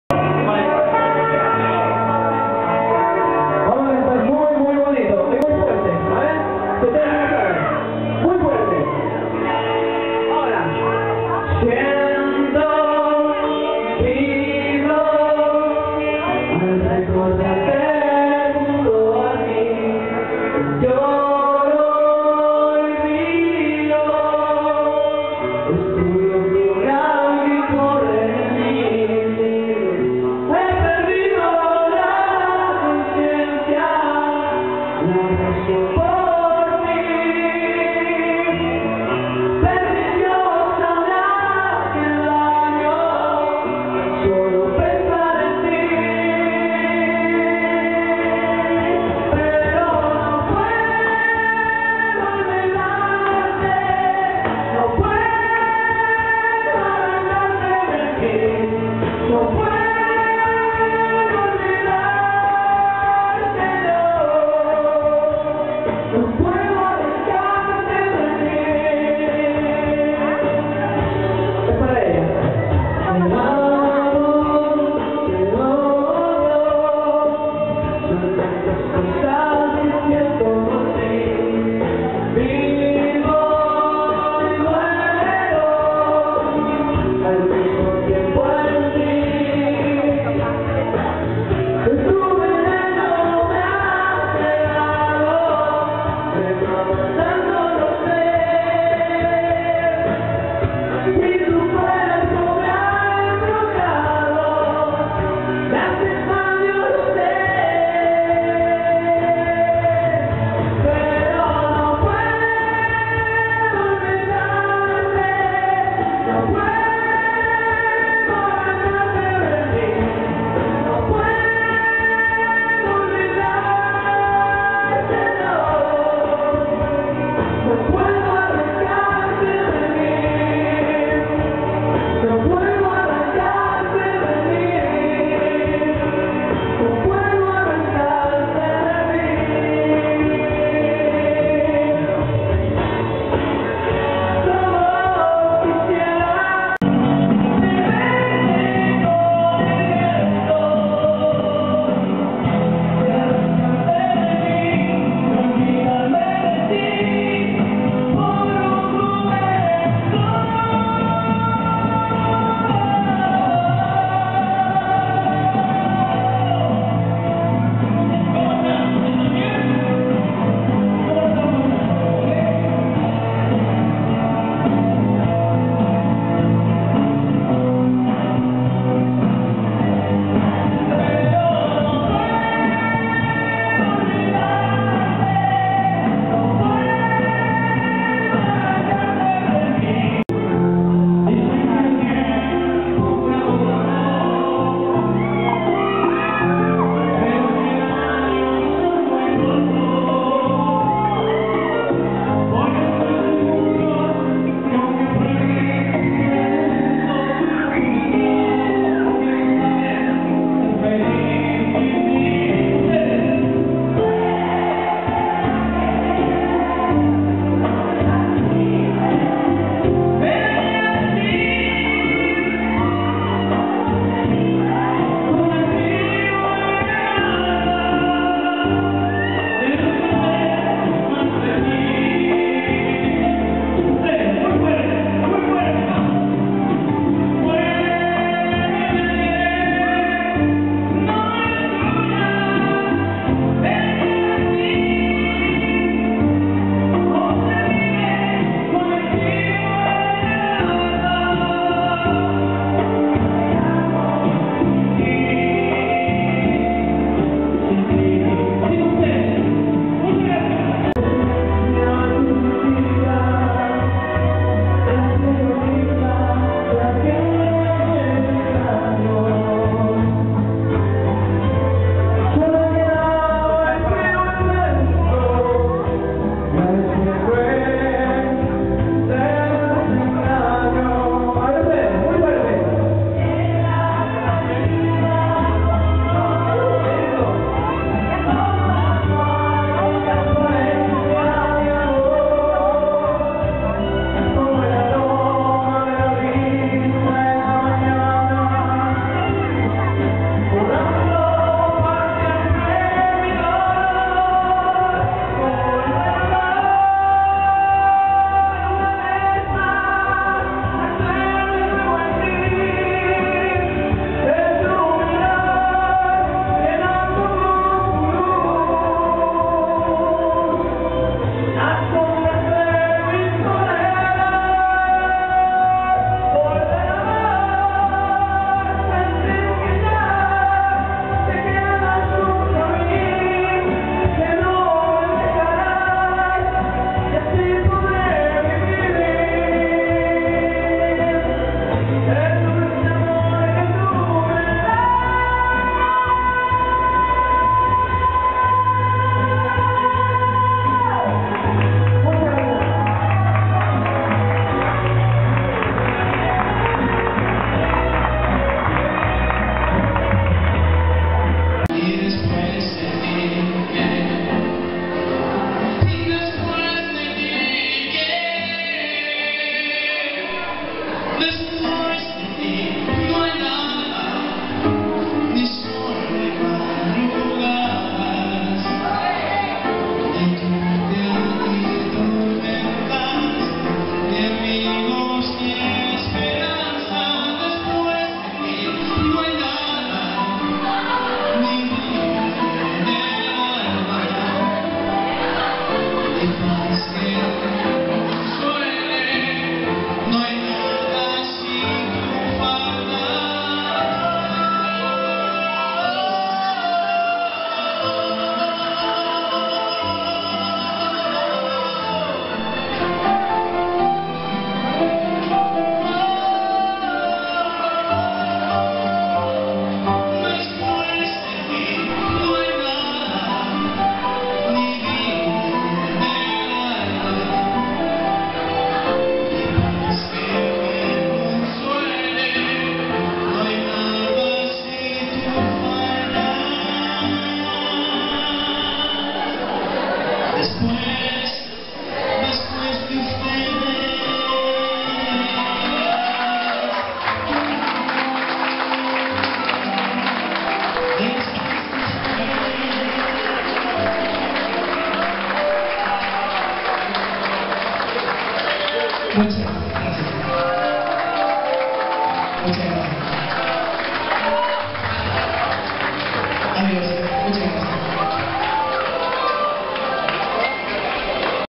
TRIBUTO CRISTIAN CASTRO CHILE Jaime Zeballos
Mix de canciones de Cristian Castro interpretado por Jaime Zeballos